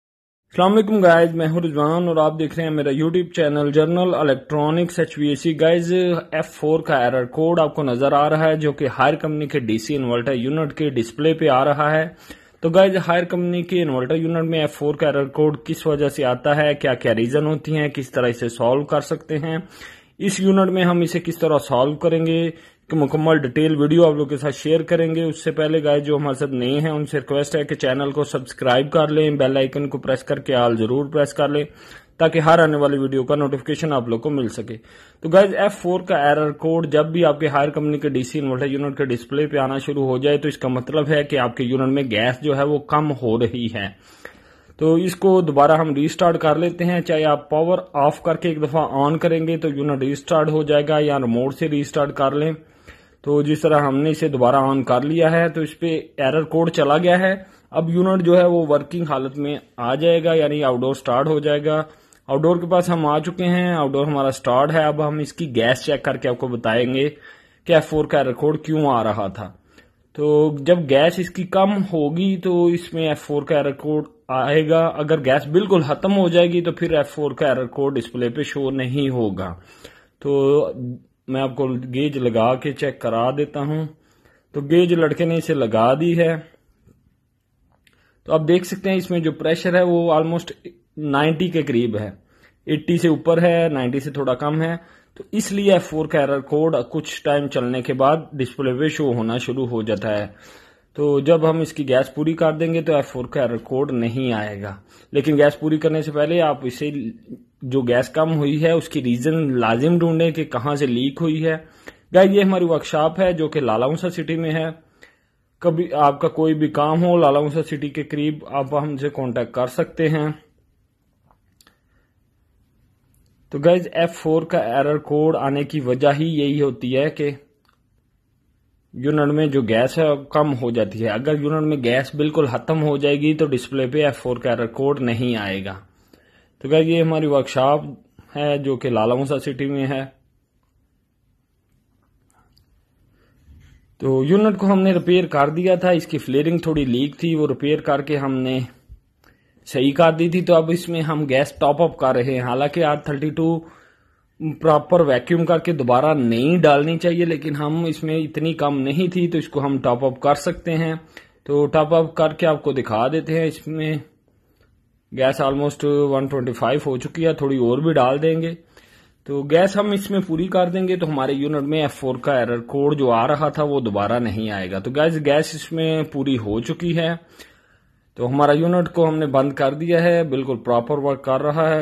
How to Remove F4 Error Code in Haier Inverter AC | General Electronics Hvac
Easy to solve F4 error code from inverter air conditioner Haier company
Inverter  ac error show on display f4 troubleshooting
General Electronics hvac